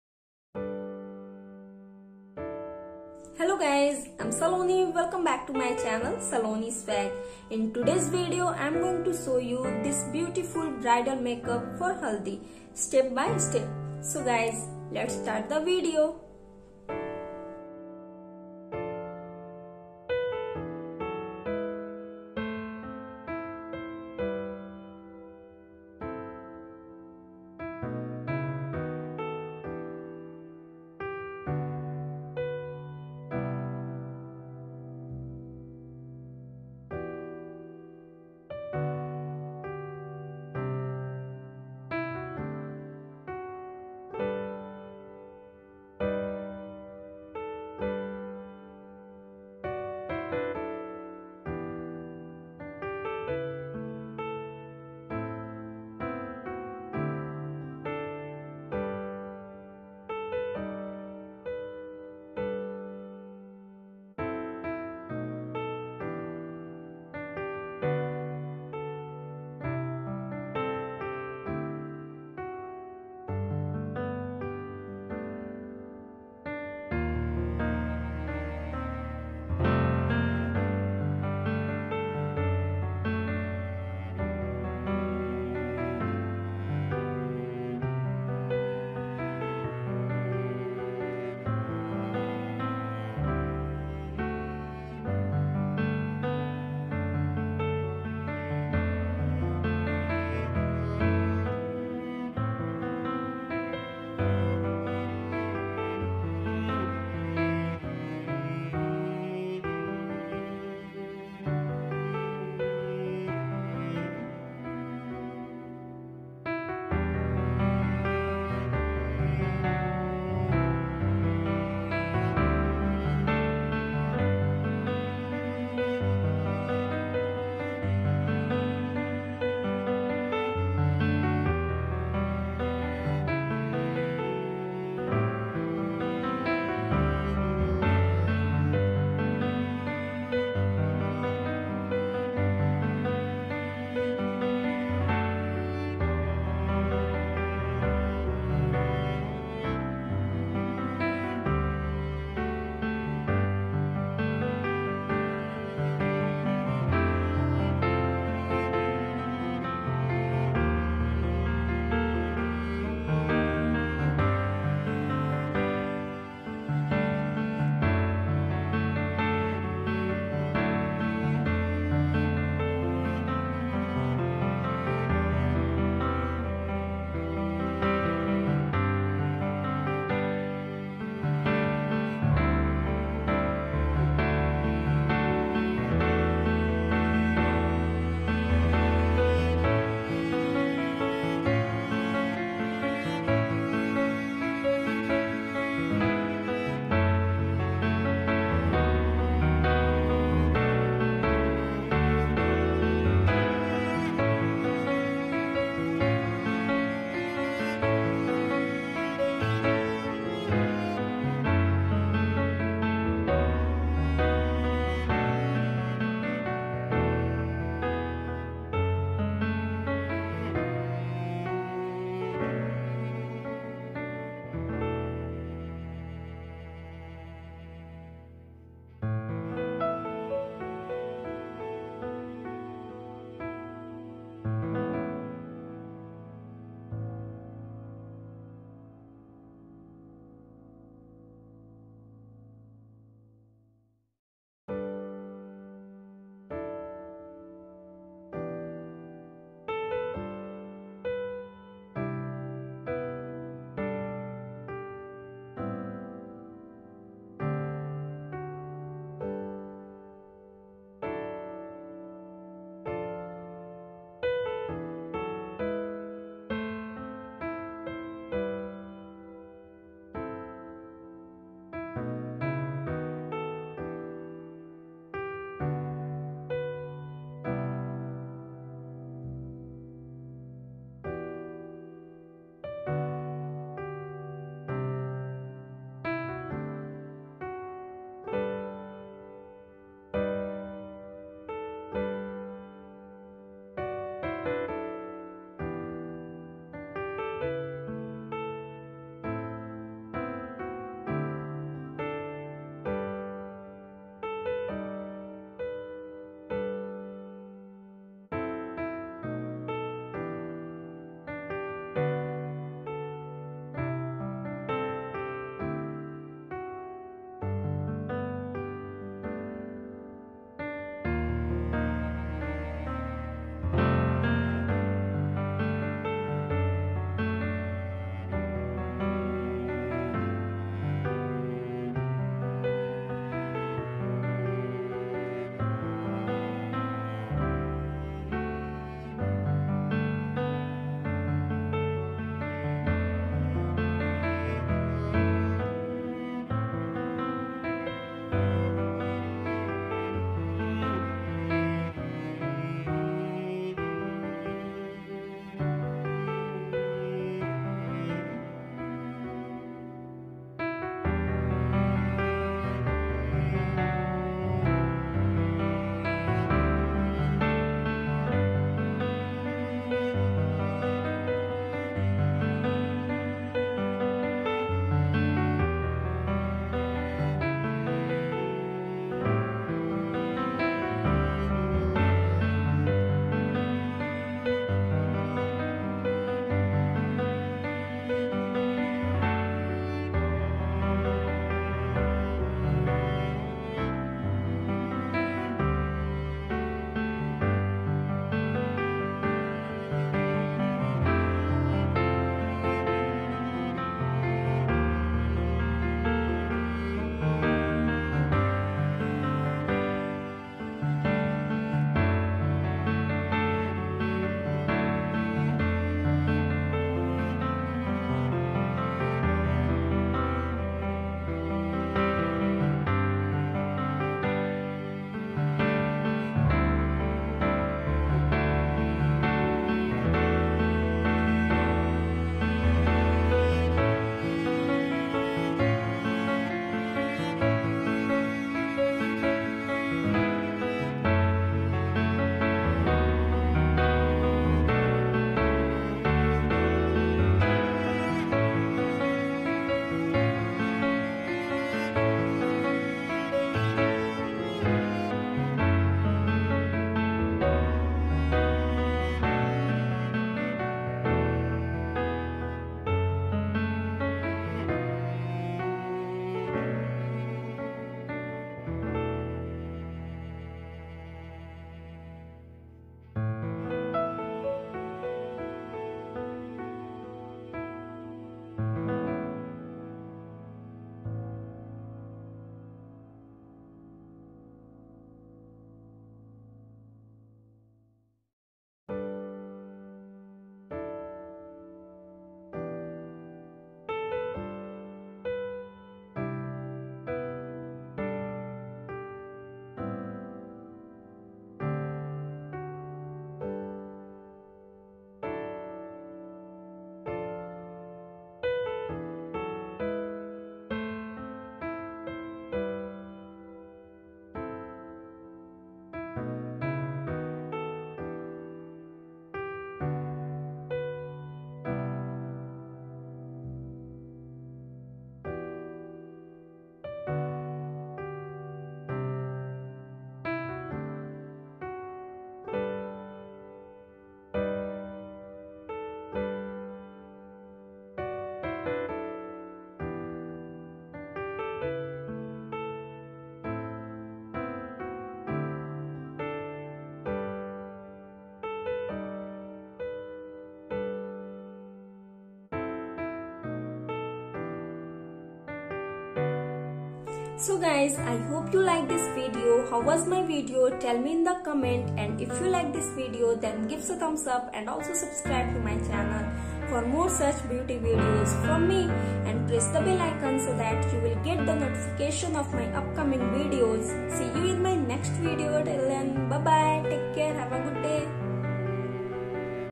Bridal Haldi Makeup look Step by Step Tutorial | New Makeup Tricks| Saloni Swag
In this video m going to show you how to do braidal makeup for haldi step by step... Easy and Best way to get ready for Haldi Function... i hope this video is helpful for you... hope u like this video..

Love ~
      Saloni Swag